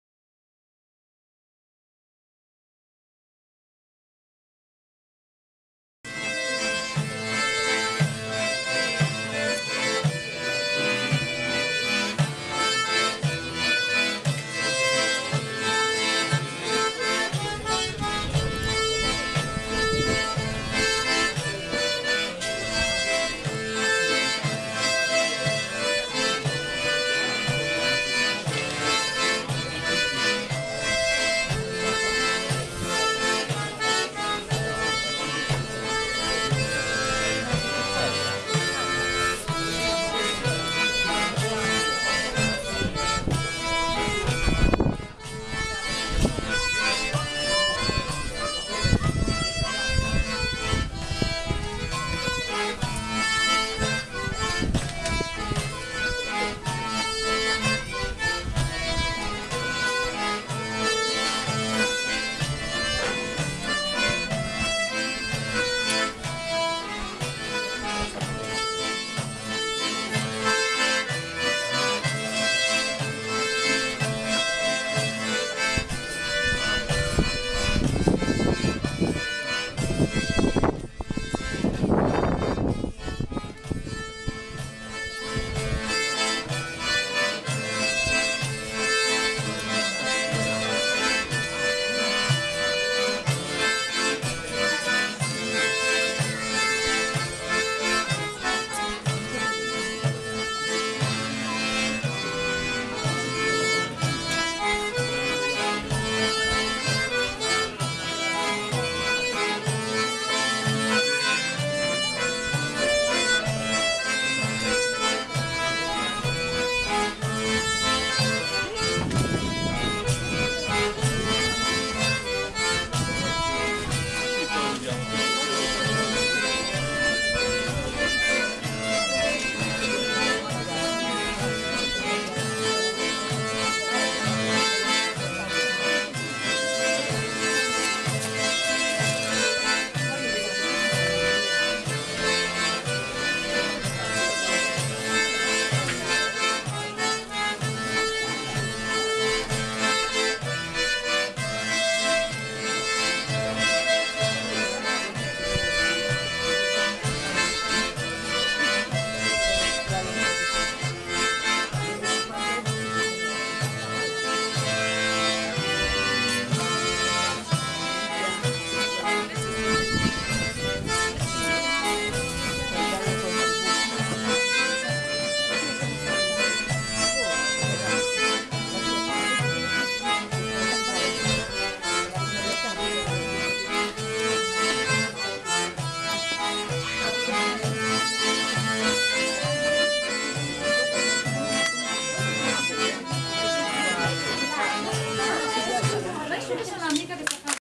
Amantici Romantici
In occasione della decima Marcia della Pace da Fragheto a Casteldelci (Rn), dopo il pranzo c'è stata l'esibizione musicale di una parte dei componenti la formazione degli "Amantici Romantici" , allievi della scuola di musica popolare di Forlimpopoli.